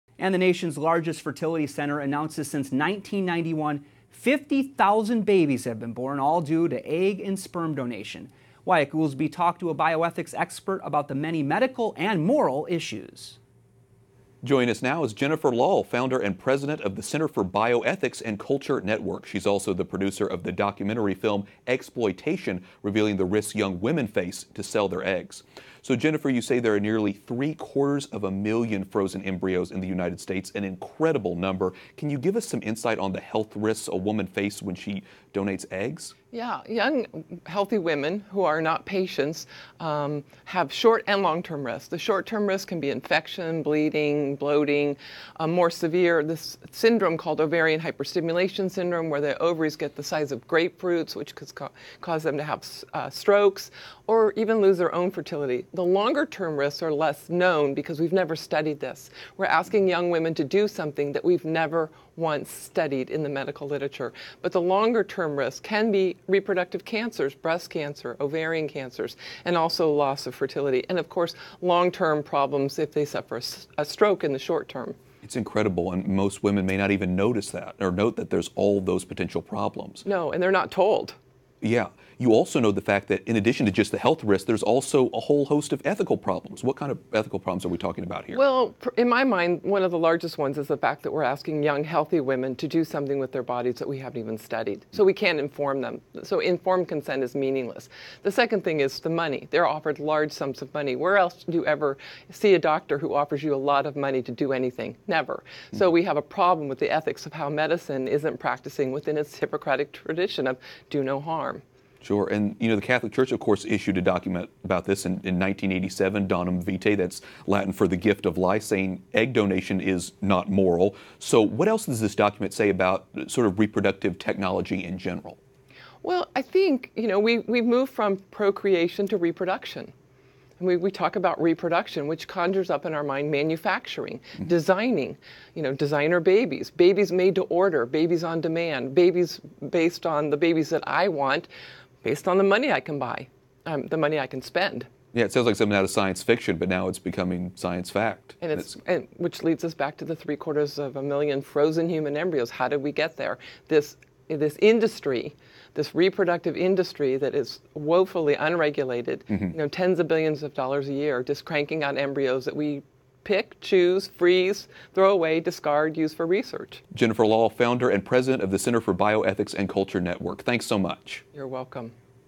“In Vitro Fertilization” and women’s health – ENN 2017-11-03
IVF expert Jennifer Lahl, a policy expert who also was a pediatric emergency nurse, explains the short-term and long-term health risks women face when they donate eggs for fertilization. It’s an important question: In the U.S. today there are more than 750,000 frozen embryos.